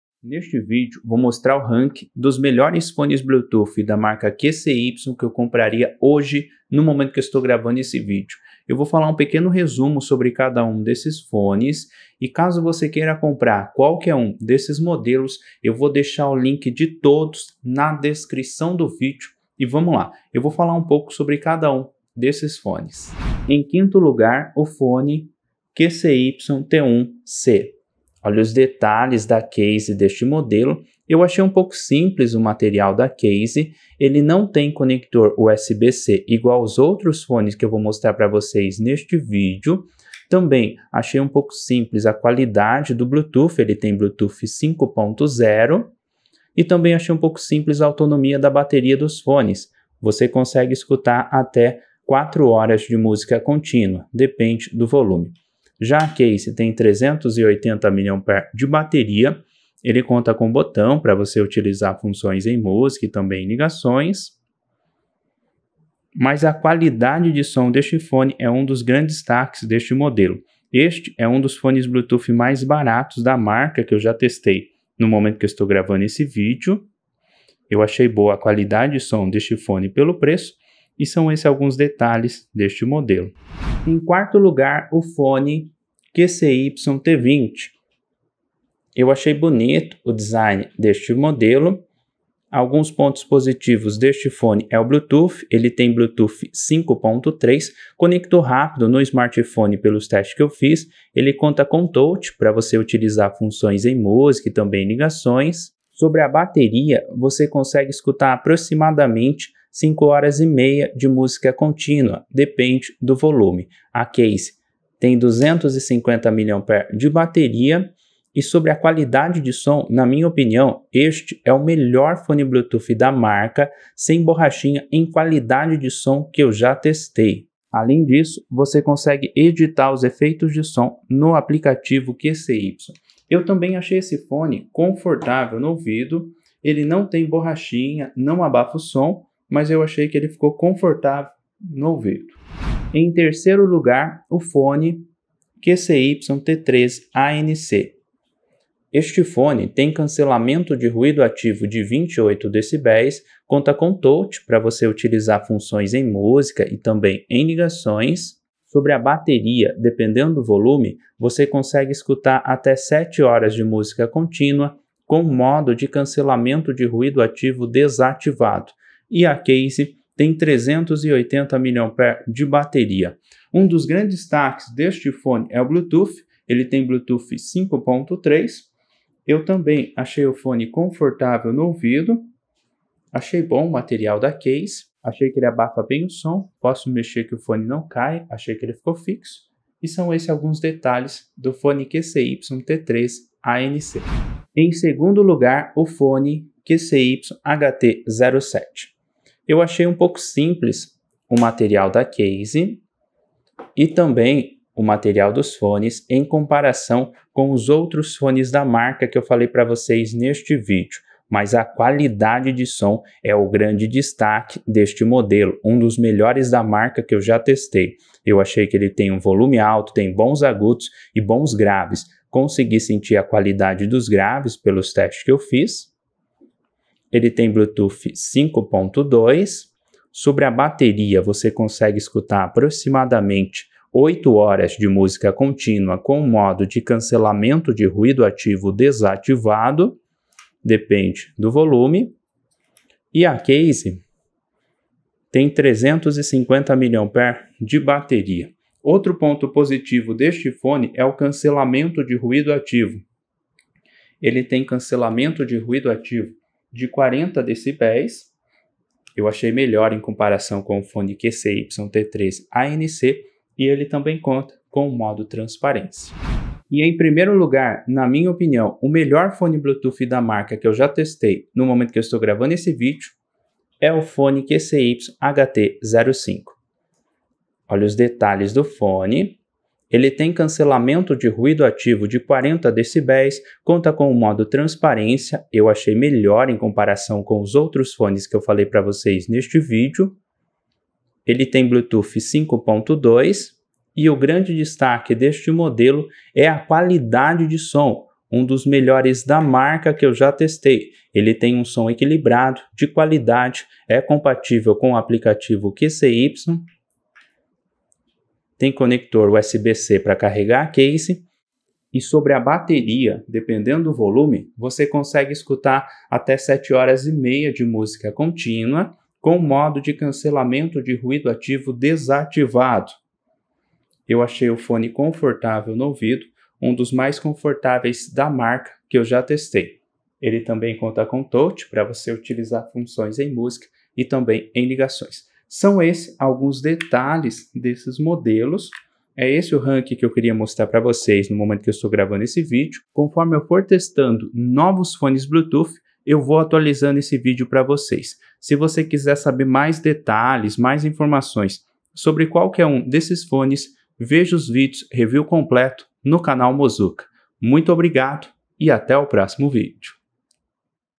RANKING 5 melhores FONES QCY 2024!
🛒 QCY T1c (Aliexpress)
🛒 QCY T1c (Amazon)
🛒 QCY T1c (Mercado Livre) (Brasil) 👉

🛒 QCY T20 (Aliexpress)
🛒 QCY T20 (Amazon)

🛒 QCY HT07 ArcBuds (Amazon)
🛒 QCY HT07 ArcBuds (Aliexpress)

(00:00) Introdução
(00:23) 5 - QCY T1c
(01:23) 4 - QCY T20
(02:22) 3 - QCY T13 ANC
(03:14) 2 - QCY HT07
(04:26) 1 - QCY HT05
(06:34) Qual tem melhor custo benefício?